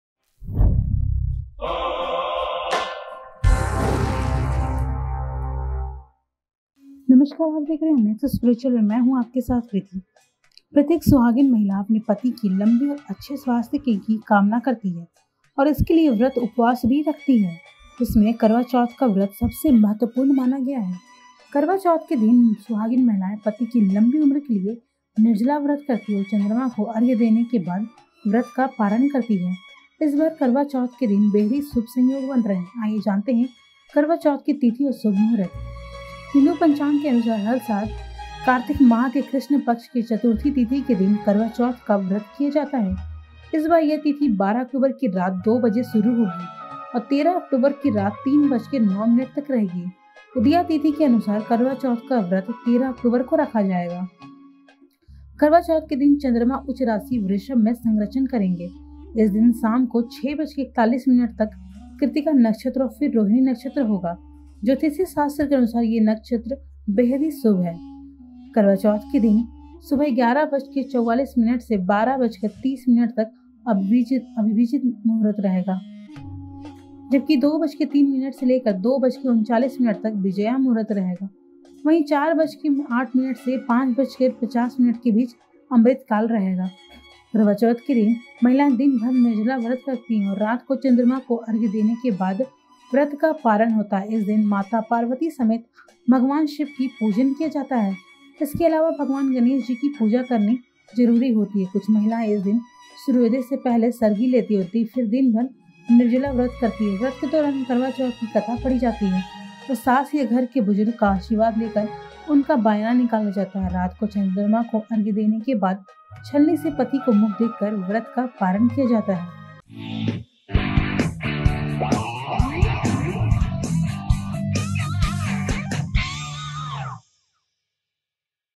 क्यों खास है इस साल का करवाचौथ, जानिए विशेष शुभ संयोग
इस बार करवाचौथ पर बनने वाले योग सुहागिन स्त्रियों के लिए बहुत फलदायी होने वाले हैं. तो चलिए जानते हैं कब कौन से संयोग बन रहे हैं.